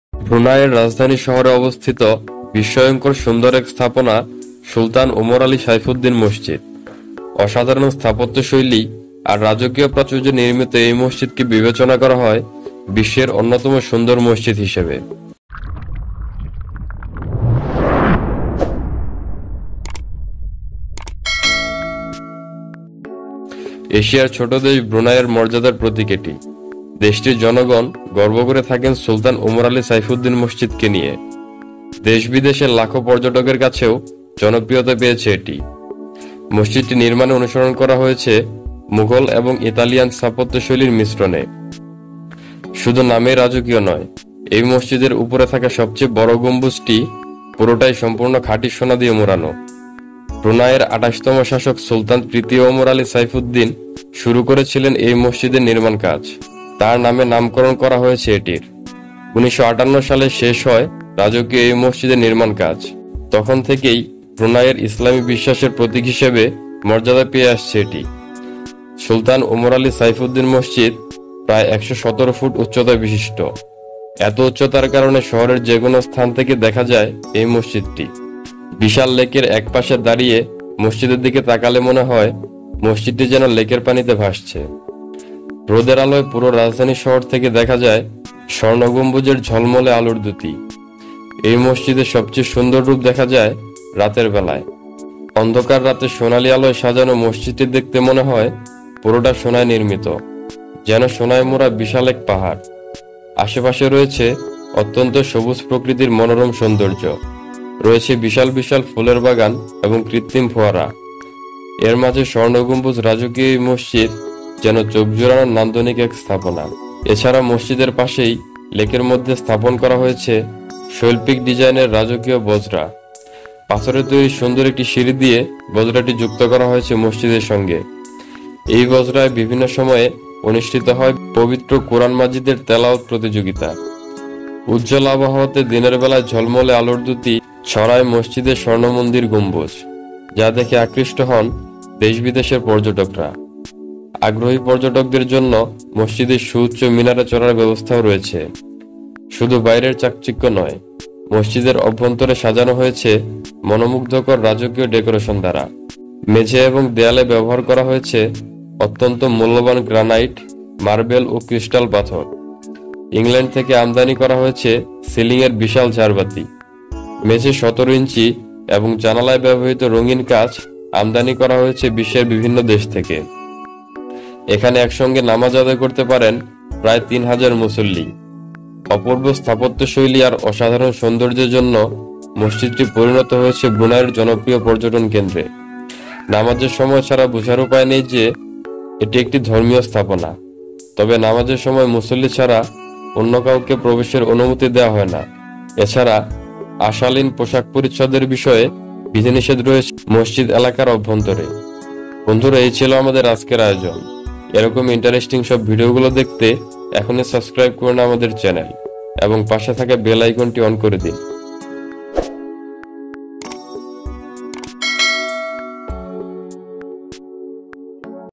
স্বর্ন দিয়ে মোড়ানো পৃথিবীর সবথেকে সুন্দর মসজিদ || Most Beautiful Mosque in the World
পৃথিবীর সবথেকে সুন্দর মসজিদ

ব্রুনাইয়ের রাজধানী শহরে অবস্থিত বিস্ময়কর সুন্দর এক স্থাপনা সুলতান ওমর আলী সাইফুদ্দিন মসজিদ। অসাধারণ স্থাপত্যশৈলী আর রাজকীয় প্রাচুর্যে নির্মিত এই মসজিদকে বিবেচনা করা হয়, বিশ্বের অন্যতম সুন্দর মসজিদ হিসেবে।

এশিয়ার ছোট দেশ ব্রুনাইয়ের মর্যাদার প্রতীক এটি। দেশটির জনগণ গর্ব করে থাকেন সুলতান ওমর আলী সাইফুদ্দিন মসজিদকে নিয়ে। দেশ-বিদেশের লাখো পর্যটকের কাছেও জনপ্রিয়তা পেয়েছে এটি। মসজিদটি নির্মাণে অনুসরণ করা হয়েছে মোঘল এবং ইতালিয়ান স্থাপত্যশৈলীর মিশ্রণে। শুধু নামেই  রাজকীয় নয়, এই মসজিদের উপরে থাকা সবচেয়ে বড় গম্বুজেটি পুরোটাই সম্পূর্ণ খাঁটি সোনা দিয়ে মোড়ানো

ব্রুনাইয়ের ২৮ তম শাসক সুলতান তৃতীয় ওমর আলী সাইফুদ্দিন শুরু করেছিলেন এই মসজিদের নির্মাণ কাজ। তার নামে নামকরণ করা হয়েছে এটির। ১৯৫৮  সালে শেষ হয় রাজকীয় এই মসজিদের নির্মাণ কাজ।  তখন থেকেই ব্রুনাইয়ের ইসলামী বিশ্বাসের প্রতীক হিসেবে মর্যাদা পেয়ে আসছে এটি।

 সুলতান ওমর আলী সাইফুদ্দিন মসজিদ প্রায় ১১৭ ফুট উচ্চতা বিশিষ্ট। এতো উচ্চতার কারণে শহরের যে কোনো স্থান থেকে দেখা যায় এই মসজিদটি। বিশাল লেকের এক প্রান্তে দাঁড়িয়ে মসজিদের দিকে তাকালে মনে হয়, মসজিদটি যেন লেকের পানিতে ভাসছে।